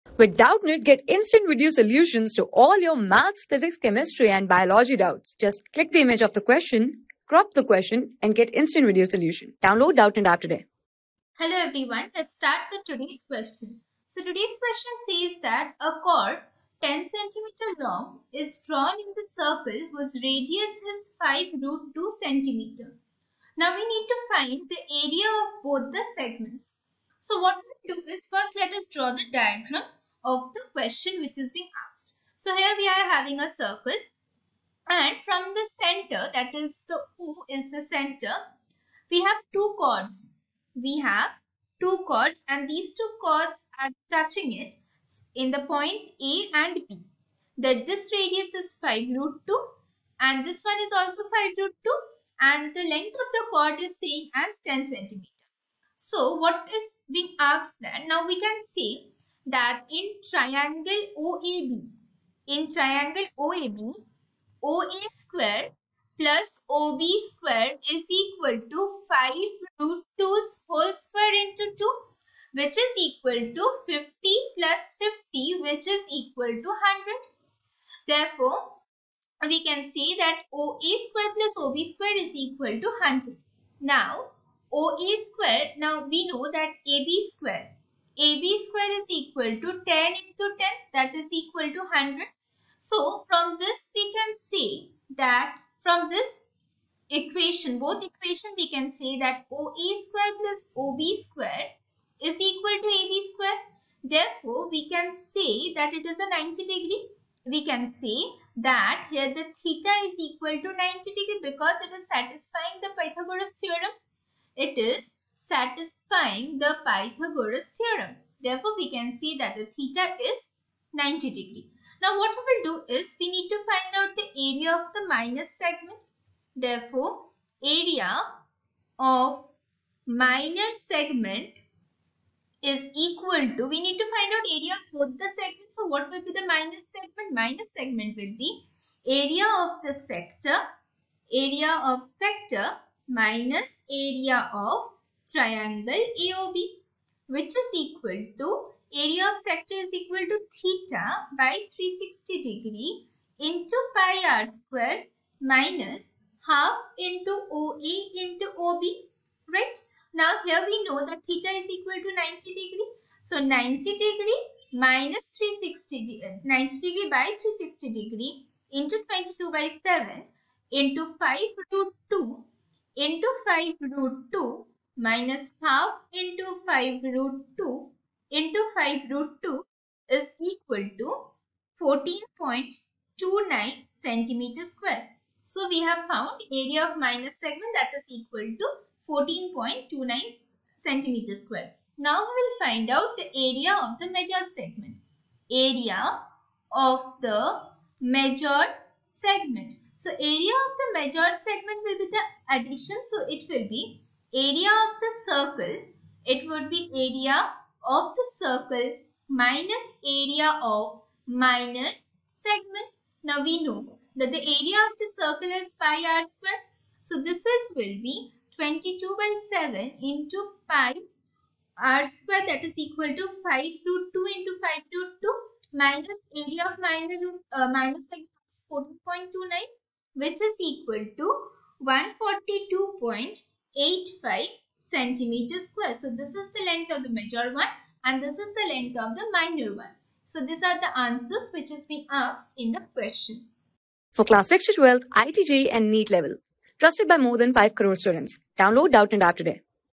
A chord 10 cm long is drawn in the circle whose radius is 5 sqrt2 cm. Find the area of both the ...
A chord 10 cm long is drawn in the circle whose radius is 5 sqrt2 cm. Find the area of both the segments
Class: 10
Subject: MATHS
Chapter: AREAS RELATED TO CIRCLES
Board:CBSE

👉 Have Any Query? Ask Us.